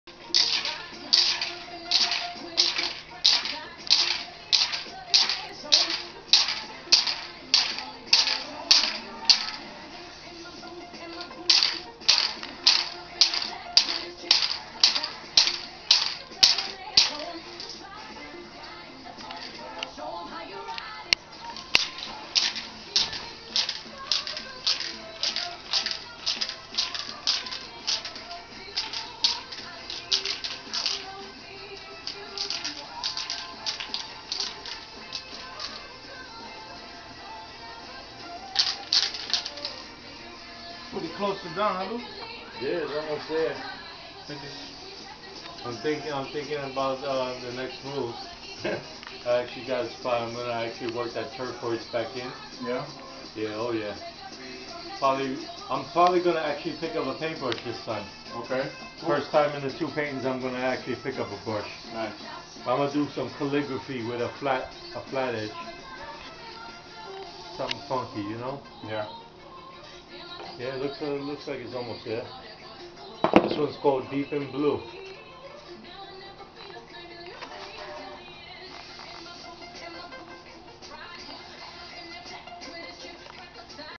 #6 sexer and zimad colabo.AVI
luis zimad lamboy and james sexer rodriguez collaboration on canvas